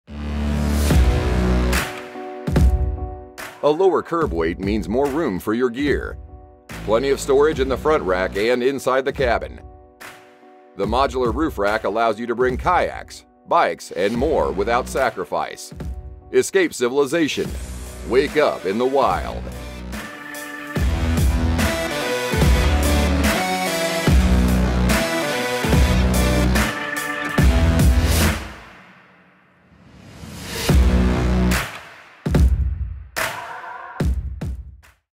All new 2021 Off Grid Trailers Expedition 2.0 Overland Trailer - Lower Curb Weight for More Gear
With the 2021 Off Grid Trailers Expedition 2.0 you can take your adventure with you.

A lower curb weight means more room for your gear. Plenty of storage on the front rack and inside the cabin.  The modular roof rack allows you to bring kayaks, bikes, and more without sacrifice.

Escape Civilization. Wake Up In The Wild